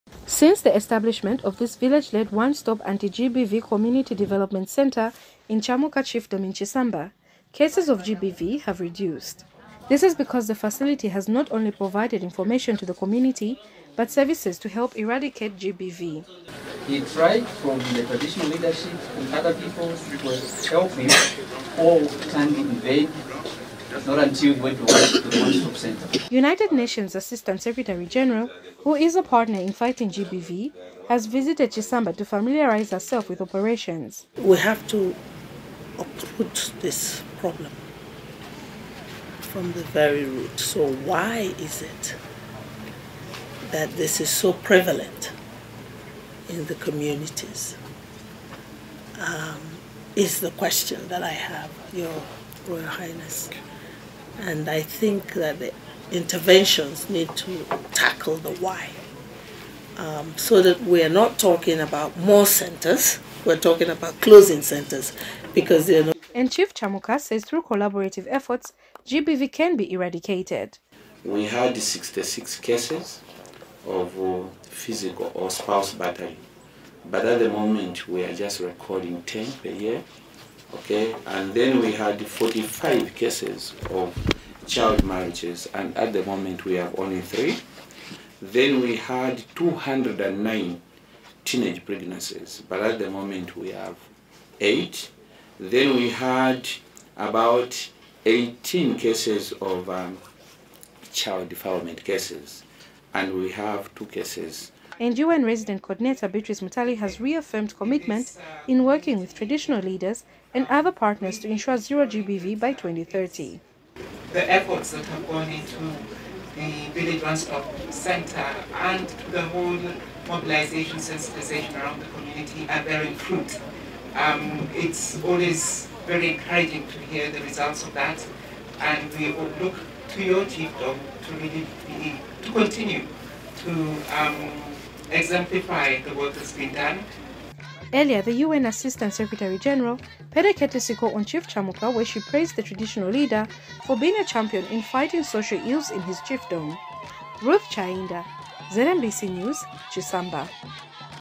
UN pledges to support GBV fight
By Ruth Chayinda

United Nations Assistant Secretary General AHUNNAK EZIAKONWA has pledged to support Zambia in fighting Gender Based Violence- GBV.

Ms. EZIAKONWA, who is also UNDP Regional Director for Africa, says the UN is committed to supporting the GBV fight and explore other areas which need urgent attention.

Speaking when she paid a courtesy call on Chief CHAMUKA at his palace in Chisamba today, Ms EZIAKONWA praised him for the role he continues to play in championing the fight against GBV against women and girls in his chiefdom.

And Chief CHAMUKA thanked the UNDP for the support it continues to offer the traditional leaders in fighting GBV and Child Marriages.

He said Zambia continues to make headway in fighting GBV through the change of laws and policies aimed at protecting the vulnerable women and girls.